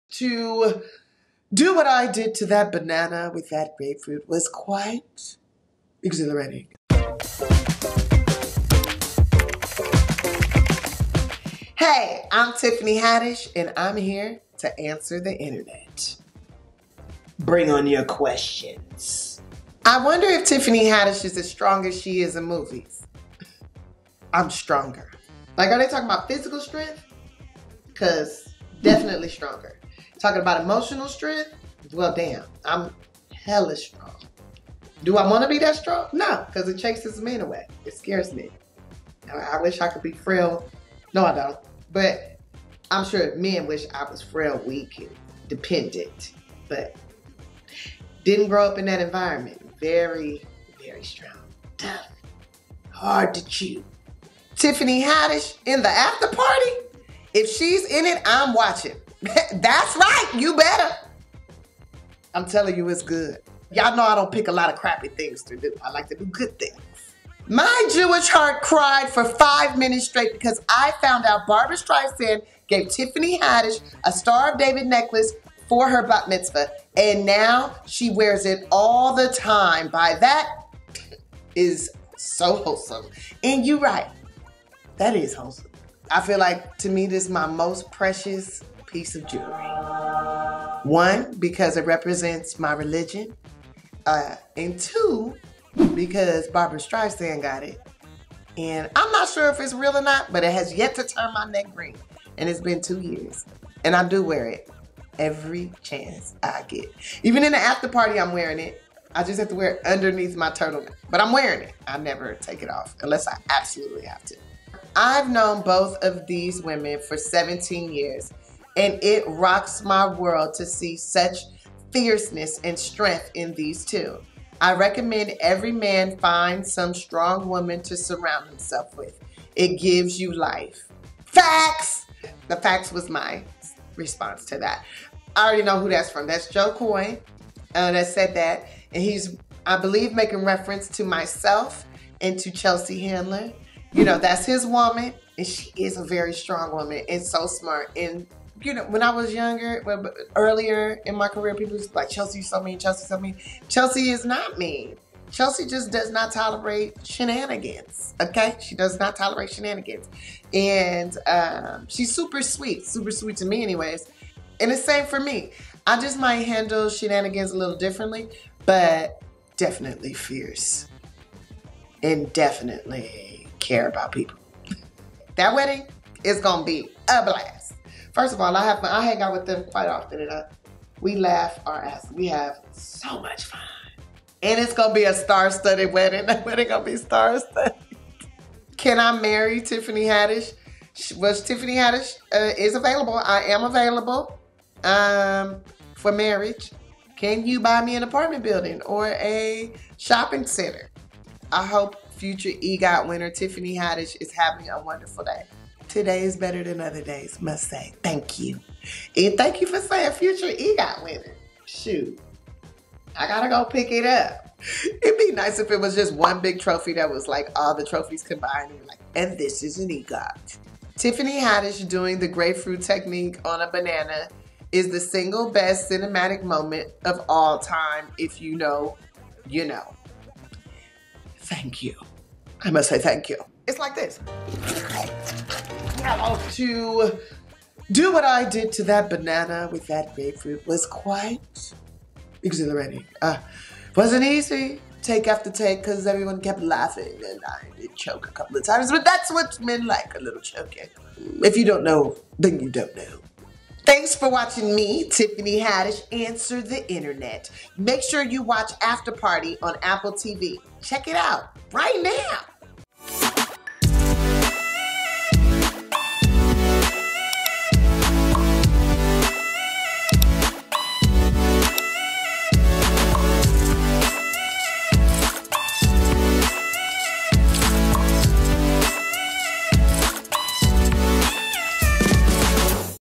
Tiffany Haddish On THAT Grapefruit Scene & Gift From Barbra Streisand
We got Tiffany Haddish to answer your stan tweets!

Find out who gifted Tiffany her most precious piece of jewellery, why she loves her bff Chelsea Handler and why filming THAT grapefruit banana scene wasn't the easiest.

Tiffany stars in The Afterparty on Apple TV+ which is out on 27th Jan!